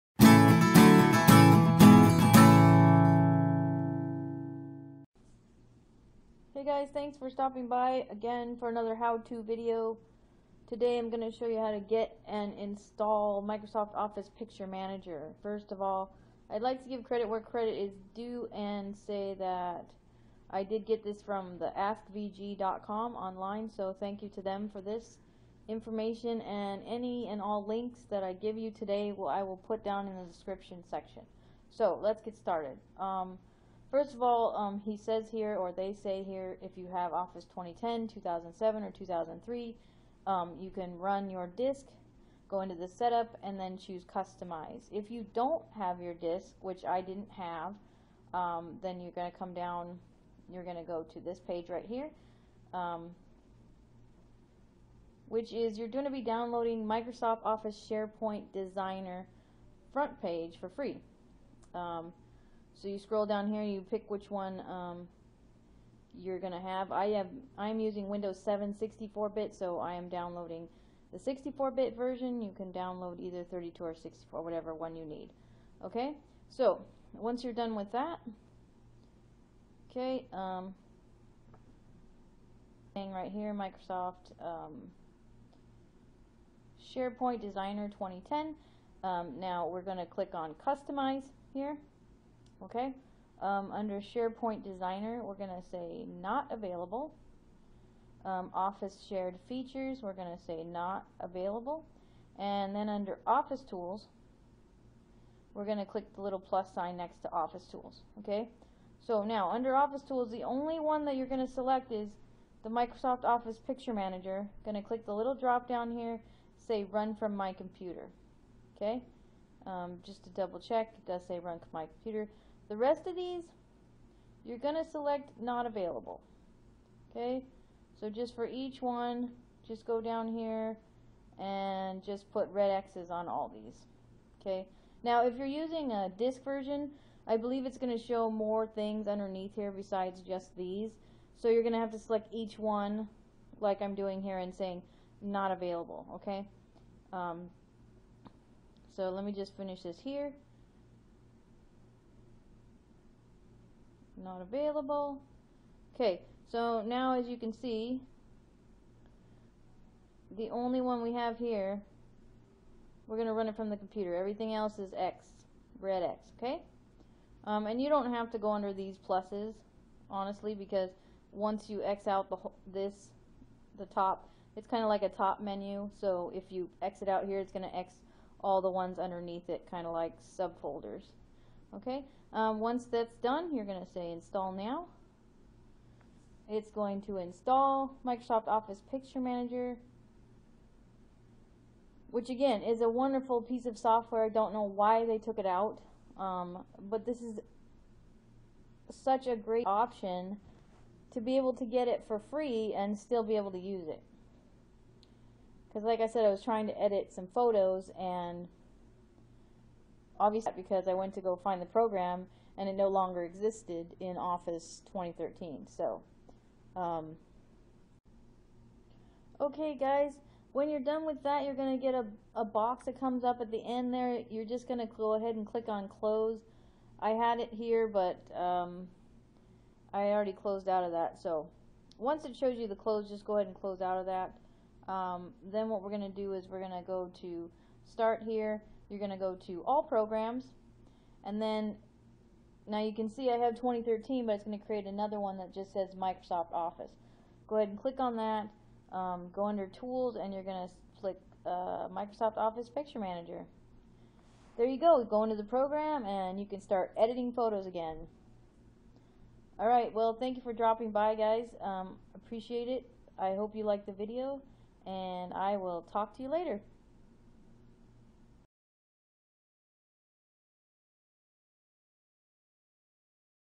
How to get and install Microsoft Office Picture Manager
This video is about how to get and install Microsoft Office Picture Manager.  It is no longer in the newer versions of Microsoft Office but still a very great picture editor.